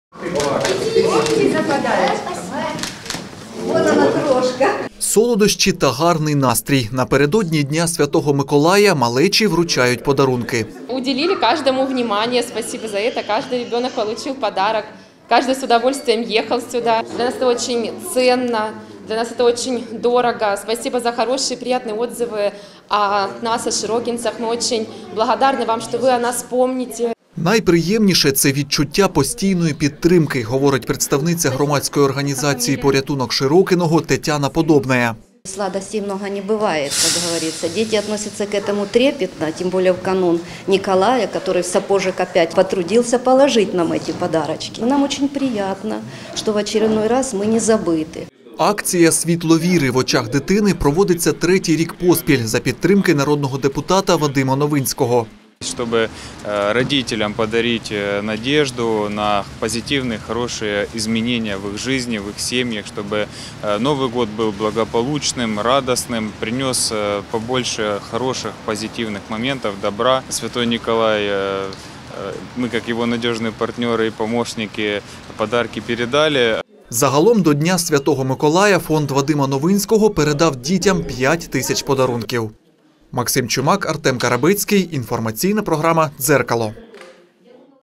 В Мариуполе дети из Широкино и малообеспеченных семей получили подарки
400 сладких сюрпризов ко Дню Святого Николая. Сегодня в Мариуполе дети из Широкино и малообеспеченных семей получили приятные подарки.